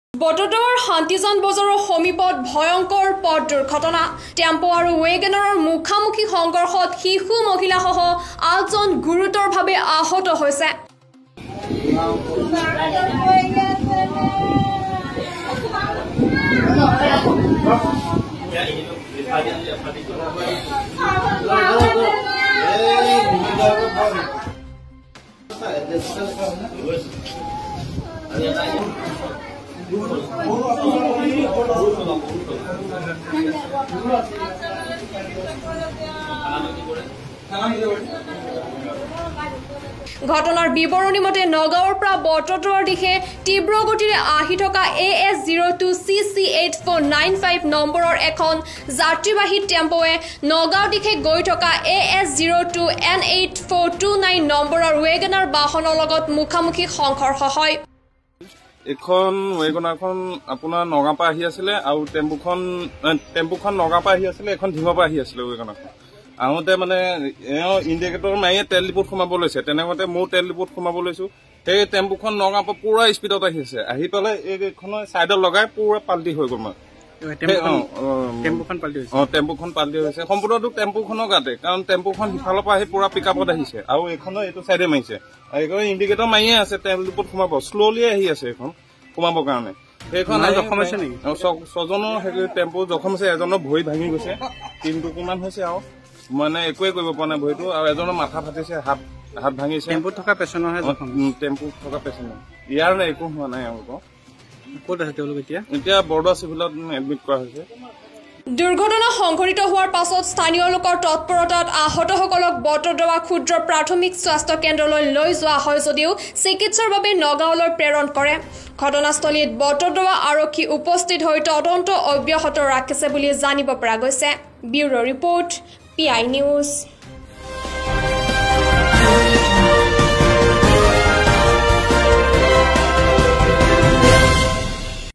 বটদ্ৰৱাৰ শান্তিজান বজাৰৰ সমীপত ভংয়কৰ পথ দুৰ্ঘটনা
বটদ্ৰৱাৰ শান্তিজান বজাৰৰ সমীপত ভংয়কৰ পথ দুৰ্ঘটনা। টেম্প আৰু ৱেগনাৰৰ মুখামুখি সংঘৰ্ষত শিশু, মহিলা সহ ৮ জন গুৰুত্বৰ ভাবে আহত।news